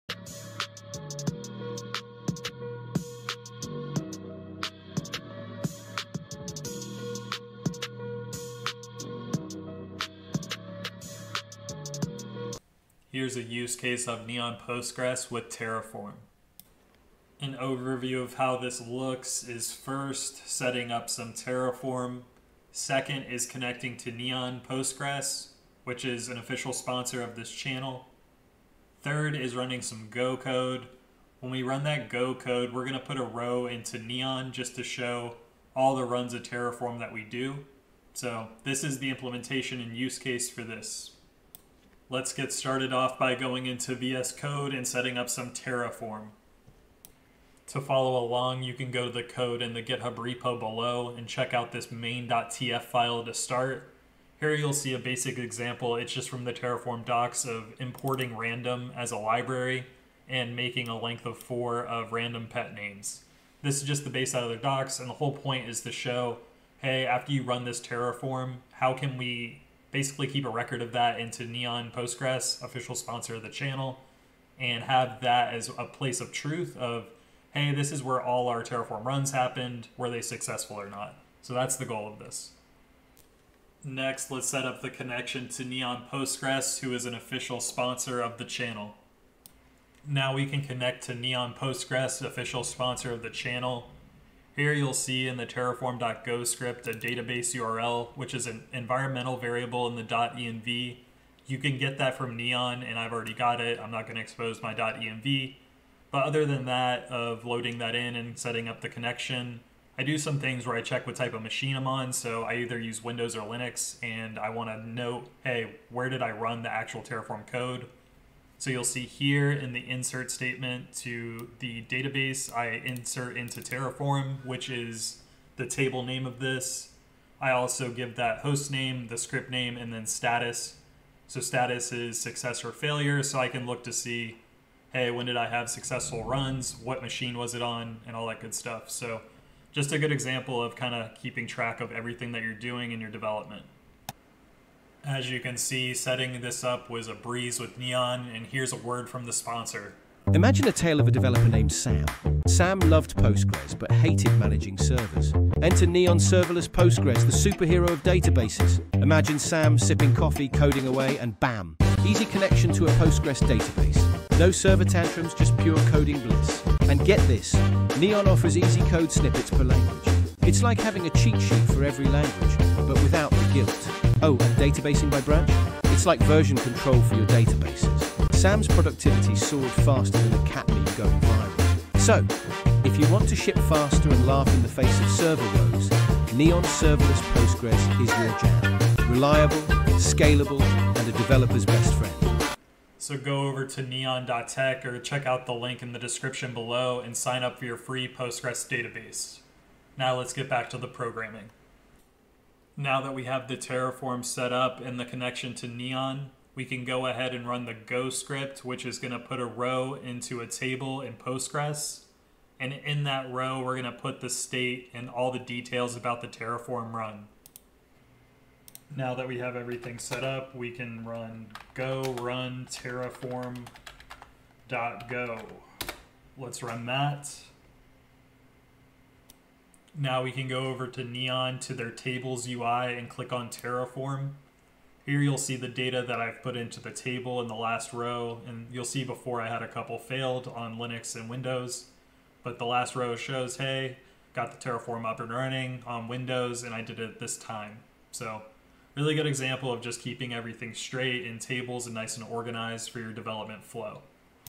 Neon Postgres for Terraform with Go 🐹
0:00 Intro
0:16 Overview
0:41 Step 1: Setup Terraform
1:23 Step 2: Connect to Neon
2:24 Sponsor
3:32 Step 3: Run Go
3:52 Neon Tables UI